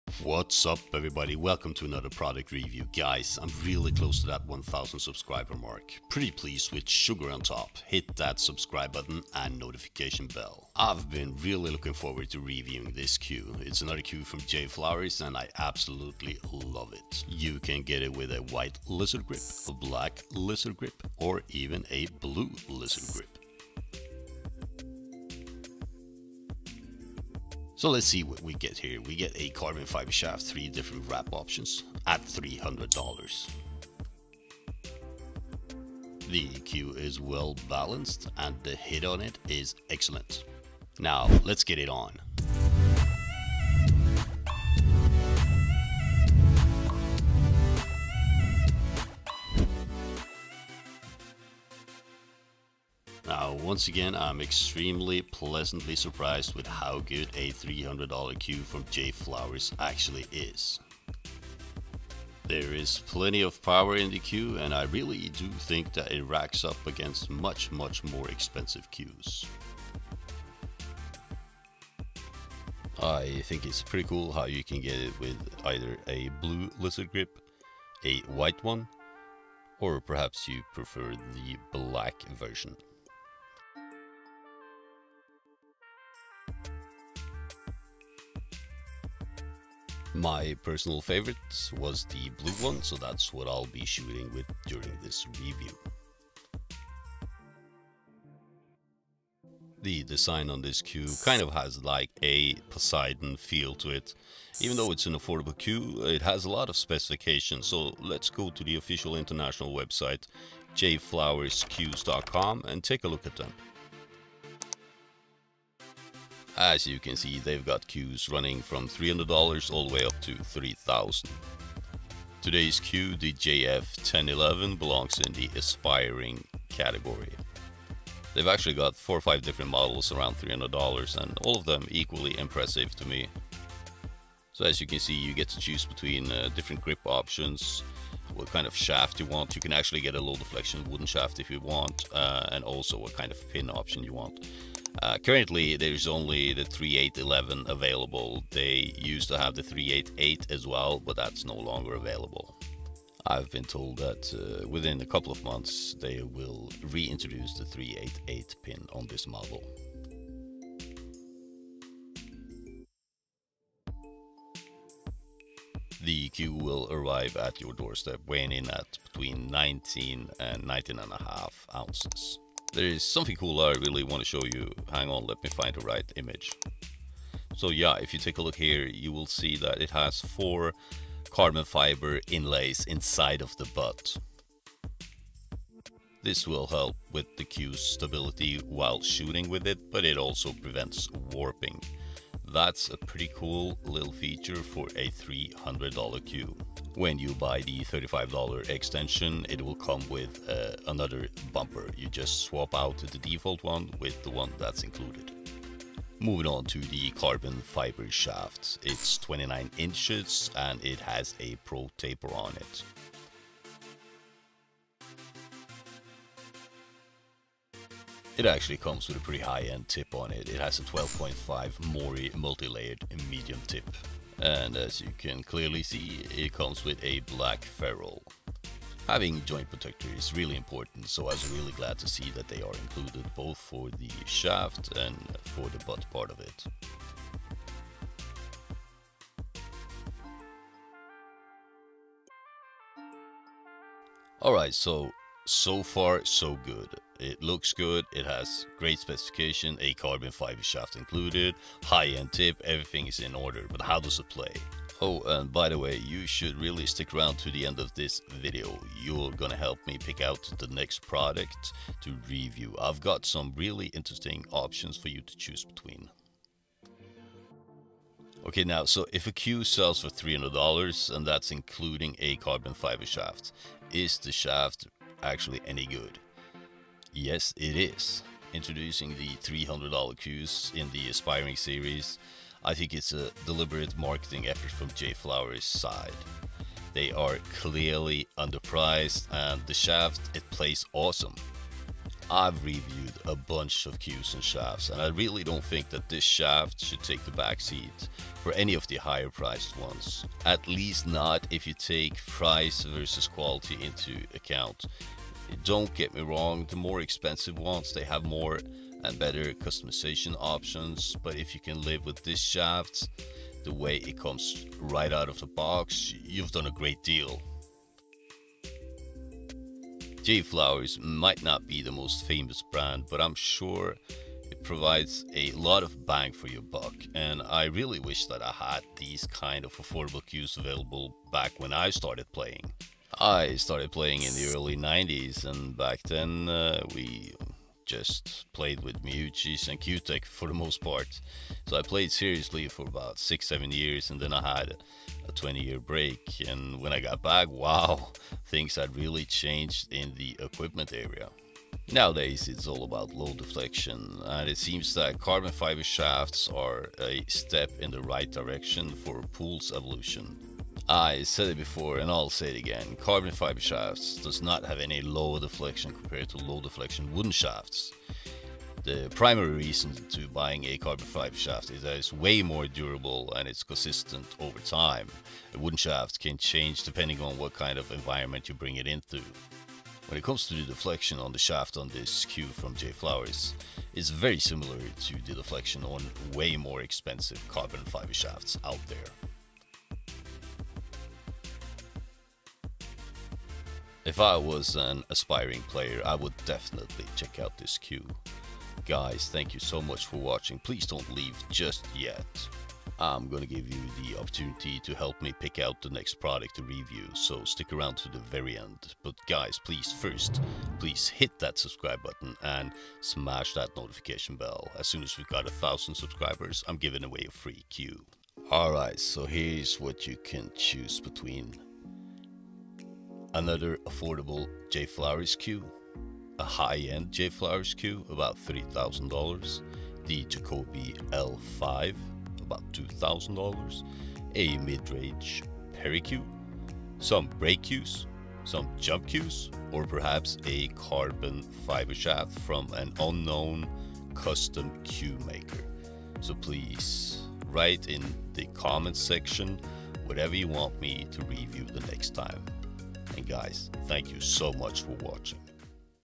Pool Cue Review: JFlowers JF 10-11 - $300 inc a carbon fiber shaft!
Only $300 including a carbon fiber shaft!? In this 2021 pool cue review, we are taking a closer look at the JF 10-11 pool cue from JFlowers.

You can choose between 3 different leather wraps and a carbon fiber shaft, or a low deflection wooden shaft.

JFlowers cues specialize in making high-quality pool cues at affordable prices. This JF 10-11 is no exception and the result from this cue review will maybe surprise you!

Shipping is 3-8 days depending on whether it's being shipped from the store in the US or directly from the factory.

00:00 Intro
00:26 JFlowers JF 10-11 review grip options and price
01:06 JFlowers JF 10-11 review close-up
01:39 JFlowers JF 10-11 review and website
03:25 JFlowers JF 10-11 review JFlowers carbon fiber shaft
04:04 JFlowers JF 10-11 review field test